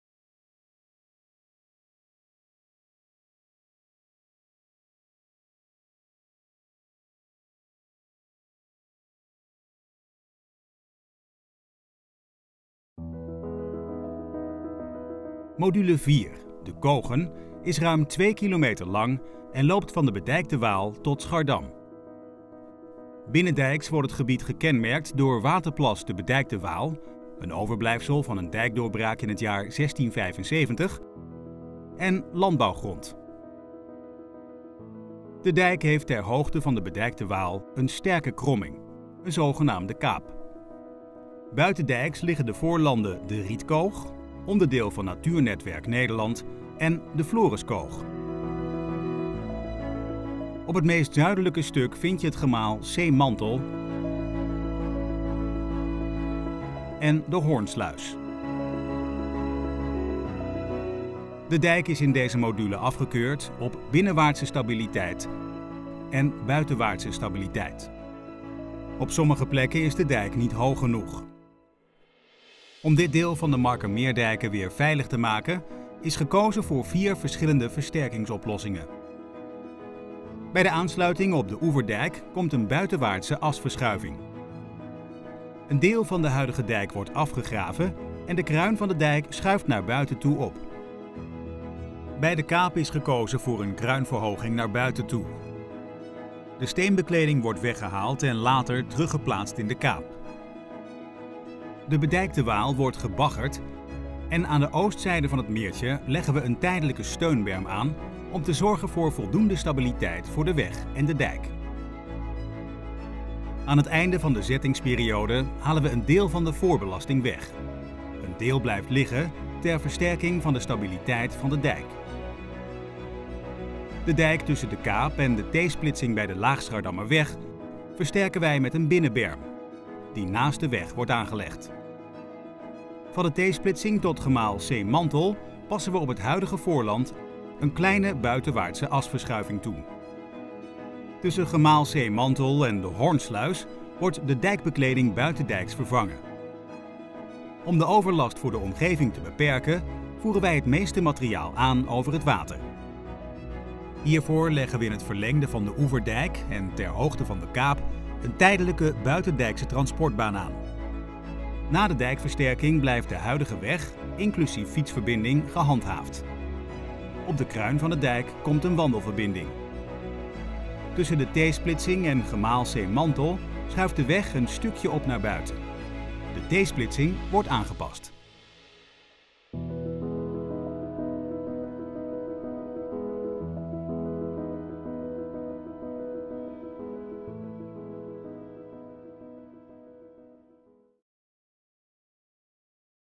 Informatieve film versterking Markermeerdijken - module 4
Module 4, De Kogen, is ruim 2 kilometer meter lang en loopt van de Bedijkte Waal tot Schardam.  Om dit deel van de Markermeerdijken weer veilig te maken, is gekozen voor vier verschillende versterkingsoplossingen.